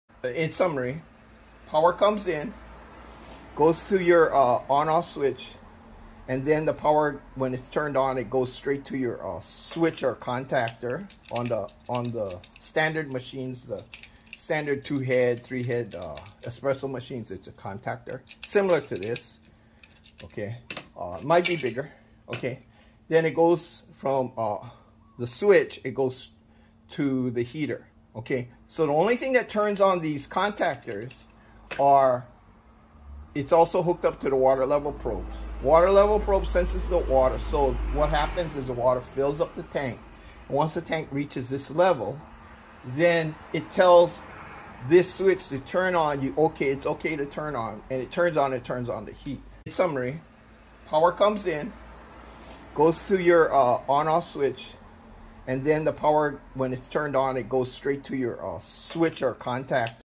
Cimbali M49, M50's Basic Boiler Heating Startup.
Power flow for all coffee espresso machines that have electric hookup will have an On/Off switch, water level control,  steam pressure switch,  Safety switch,  High limit switch,  and sometimes a critical temperature control.   Gas fired systems use a sight glass with manual boiler filling.  Gas heater is manually fired up and steam pressure monitoring by pressure guage.  Modern systems eliminate the need to constantly monitor pressure,  though remnants of those models persist in with pressure and sight water guages still being included.   Guage s still adds a flair of credibility to a models with these old  style devices.
1.  Input utility power at 110-120VAC 15 AMPS or 200 to 240 VAC 30 AMPS,  Both at  Single (1) phase
2. On off power switch.
3. Boiler water level control.   Checks water level and refills tank as needed.
4. With water filled.  Water board passes control to steam pressure switch.
5. Boiler pressure is monitored by pressure switch.   with no pressure it will be on to request heater elements to turn on.
6. heater contactor activates to supply AC voltage to element(s).
7. Steam pressure monitored for correct pressure setting.  Usually at 1.2 BAR
8. Steam switch now directly controls the heating of boiler at this point.